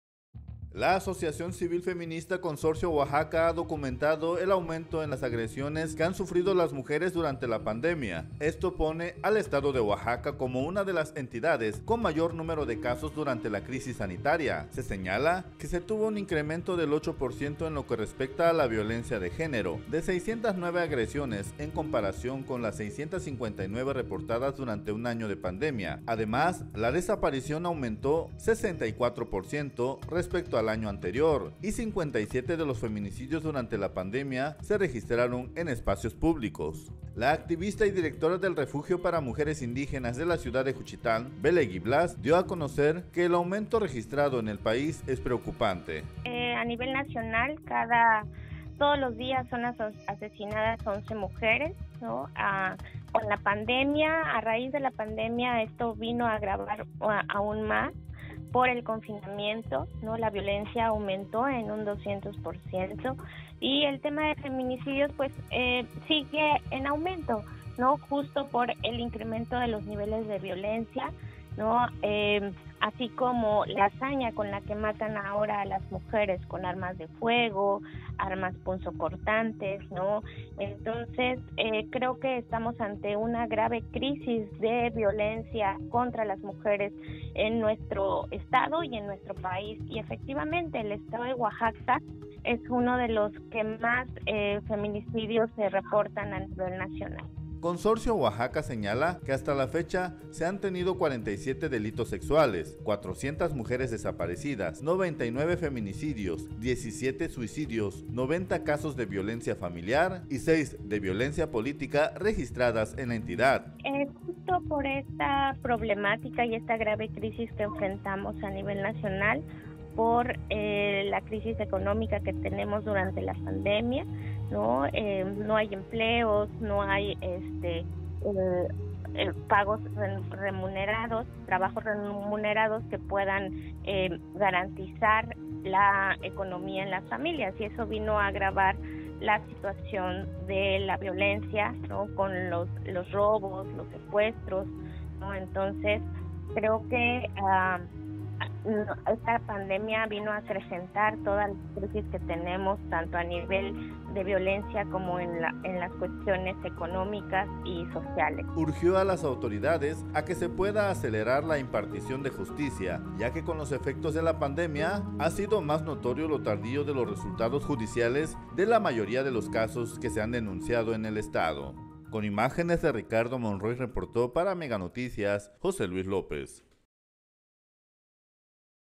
NP FEMINICIDIOS OAXACA
La asociación civil feminista Consorcio Oaxaca, ha documetado el aumento en las agresiones que han sufrido las mujeres durante la pandemia, esto, pone el Estado de Oaxaca en una de las entidad con mayor número de casos durante la crisis sanitaria.